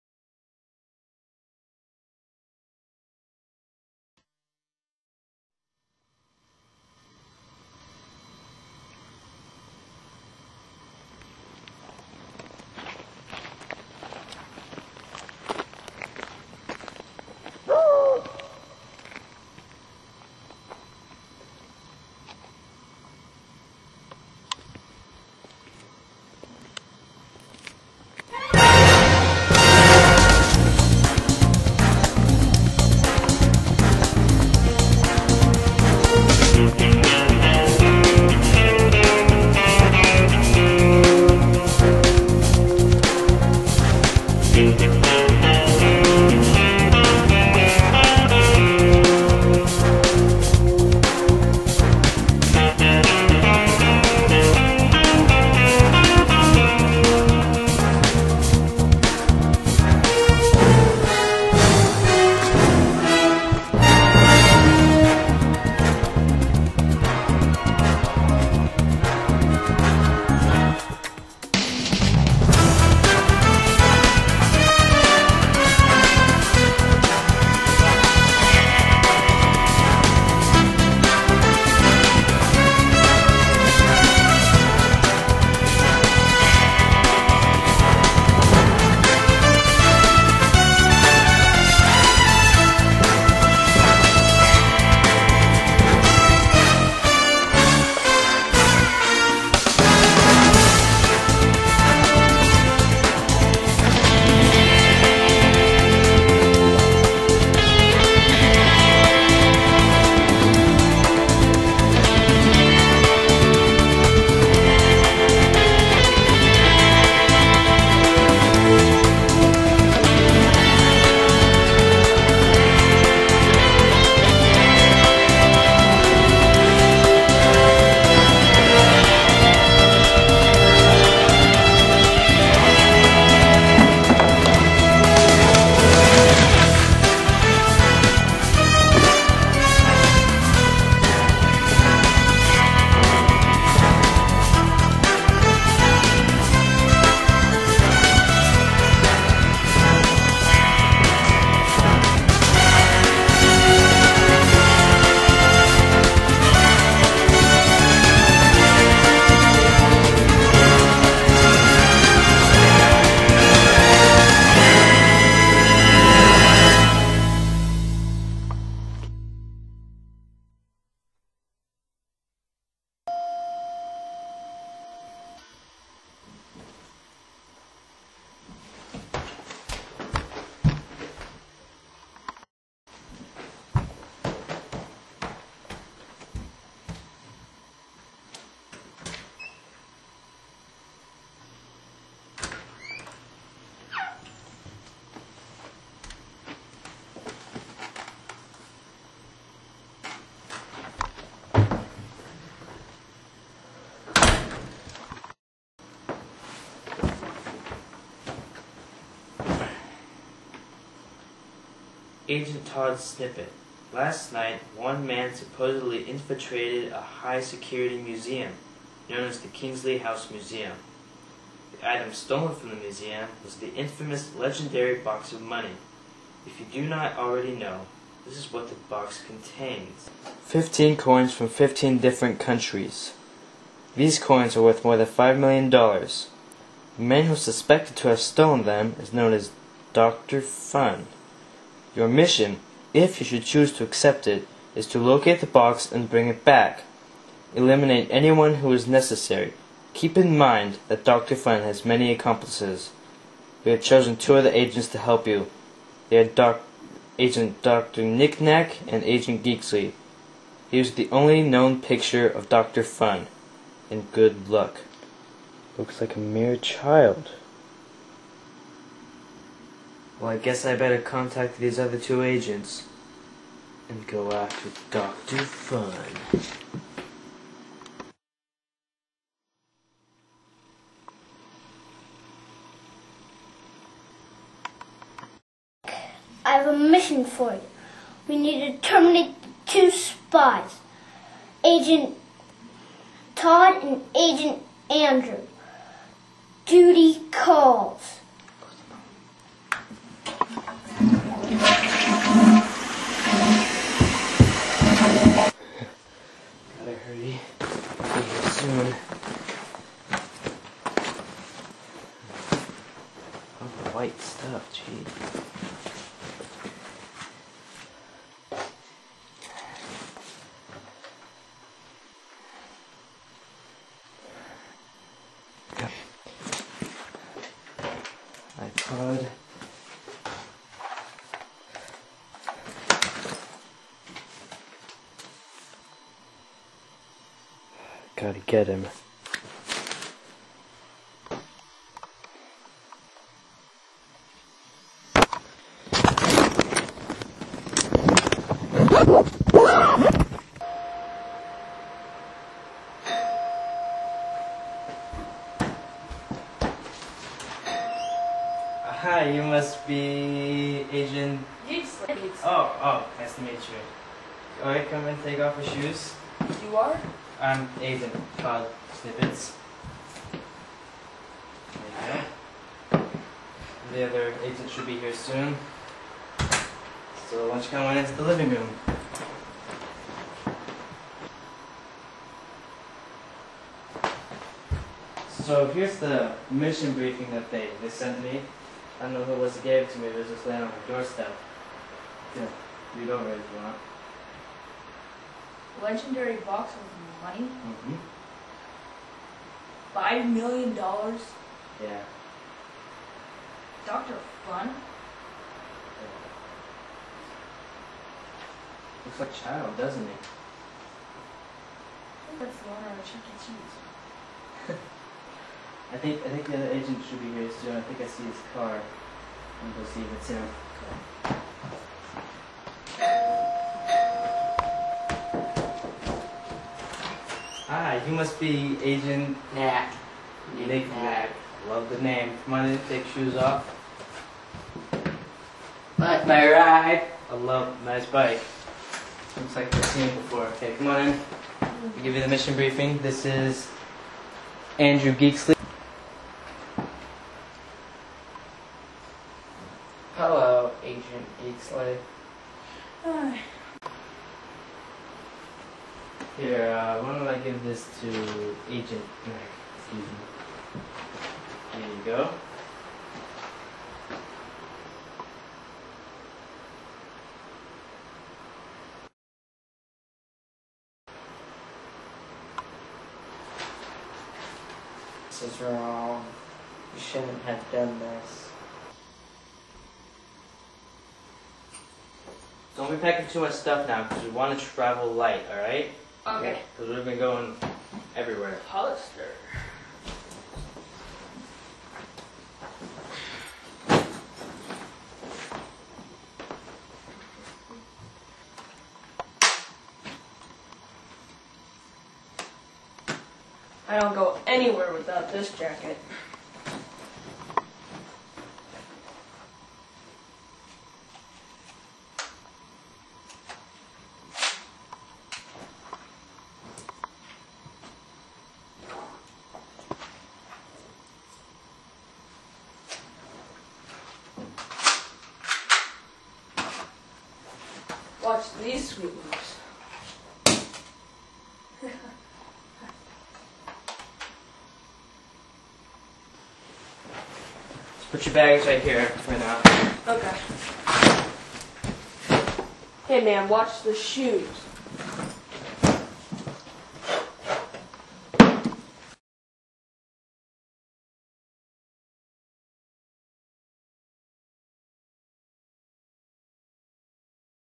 Not So Secret Agents 1 (part1)
The evil Dr. Fun has just stoled jewels from a museum. And it is now up to 3 unlikly secret agents to capture Dr. Fun and return the jewels. Are They up to the challenge?